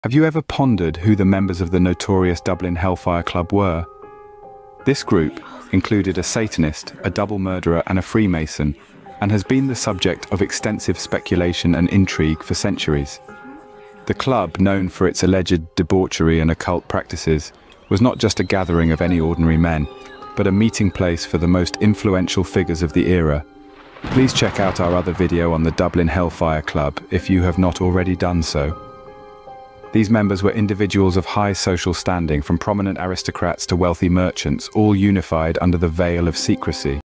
True Crime: The Notorious Dublin Hellfire Club!
Discover the truth behind the enigmatic members of the Dublin Hellfire Club, including Satanists, murderers, and Freemasons. Dive into the centuries-old speculation and intrigue surrounding this secretive group known for their alleged debauchery and occult practices. Join us in unraveling the mysteries of this infamous gathering of influential figures. Don't miss our other video on the Dublin Hellfire Club for more fascinating insights.